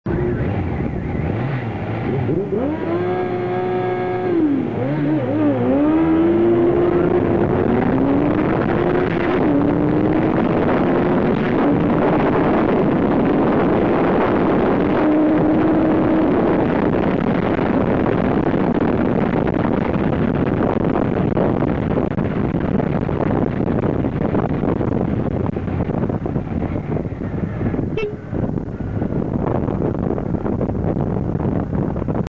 Honda Blackbird sprint North Weald
On board video of my Blackbird doing a run down the 1/4 mile strip and North Weald. Best time was 10.75 @ 129 MPH into a 10 MPH head wind. Temperature was around 19ºC.